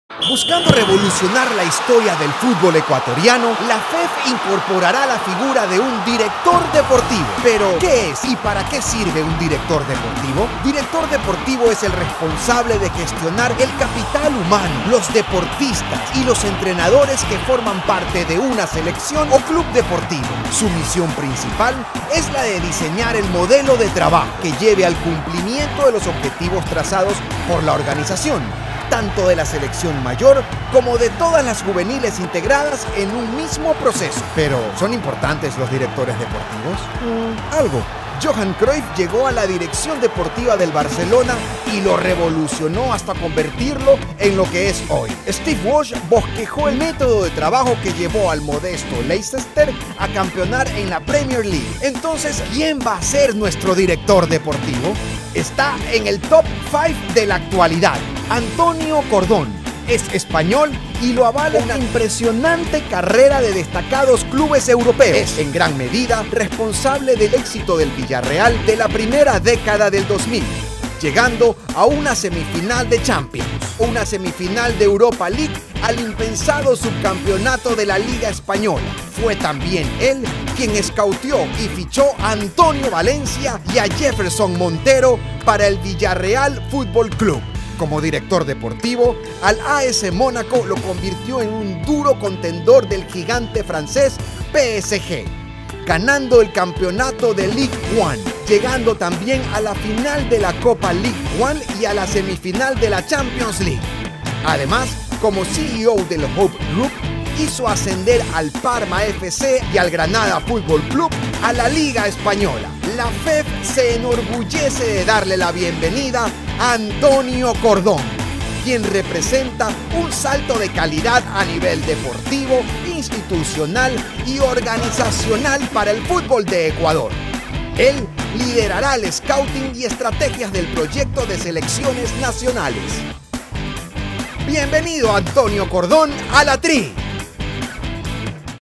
¡Bienvenido, Antonio Cordón! Nuevo Director Deportivo de la Selección
✔ Antonio Cordón 🇪🇸
✔ Edad: 56 años
✔ Fue Director Deportivo en el @VillarrealCF y @AS_Monaco. 🔝
✔ Fichó a Antonio Valencia y a Jefferson Montero en el Villarreal cuando dieron su salto a Europa 🌍

Ahora en LaTri ⚽ :

✔ Liderará scouting y el Proyecto de Selecciones Nacionales
✔ Desarrollará el área de Sport Science & Analytics, para convertirla en un modelo a seguir para los clubes del futbol en Ecuador, y de esta manera elevar la vara en lo referente al fútbol local.
📋 Con esta contratación de talla mundial, la FEF culmina un año arduo de preparación del proyecto estratégico que nos permitirá dar el salto de calidad necesario.